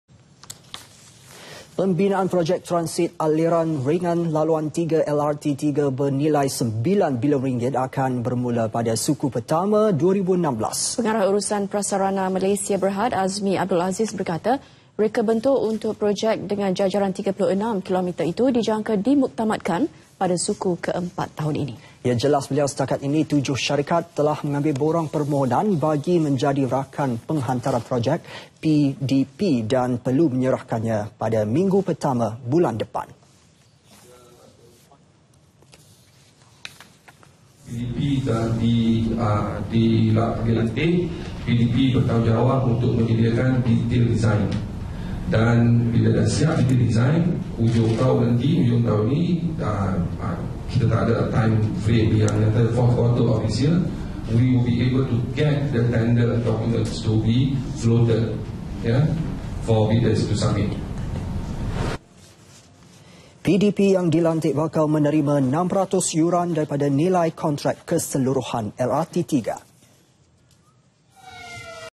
PEMBINAAN LRT 3 BERMULA 2016 [12 MEI 2015]
Pembinaan projek Transit Aliran Ringan Laluan 3 ( LRT3) bernilai 9 bilion ringgit akan bermula pada suku pertama 2016.

Pengarah Urusan Prasarana Malaysia Bhd, Azmi Abdul Aziz berkata reka bentuk untuk projek dengan jajaran 36 kilometer  itu dijangka dimuktamadkan pada suku ke empat tahun ini.

VD: ]]

Jelasnya setakat ini 7 syarikat telah mengambil borang permohonan bagi menjadi Rakan Penghantaran Projek, PDP dan perlu menyerahkannya pada  minggu pertama bulan depan.